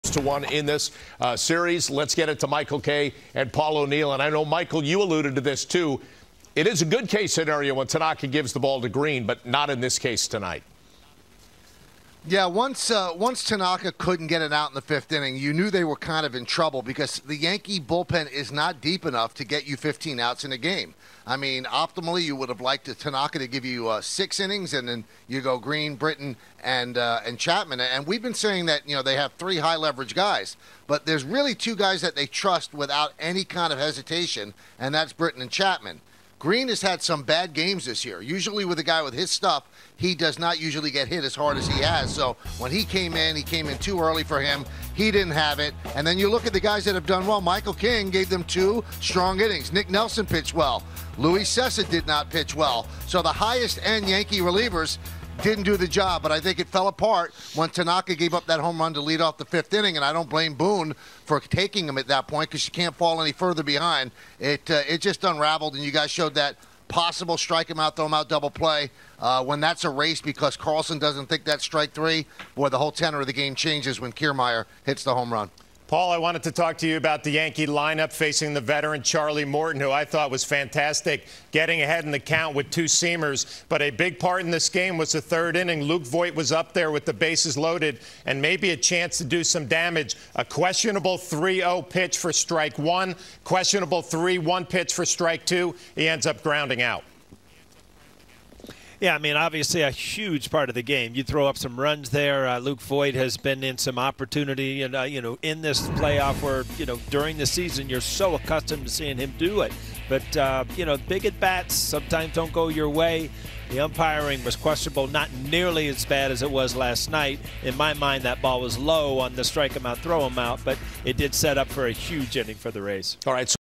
ALDS Game 3 loss reaction
Michael Kay and Paul O'Neill react to the Yankees 8-4 loss to the Tampa Bay Rays in Game 3 of the ALDS.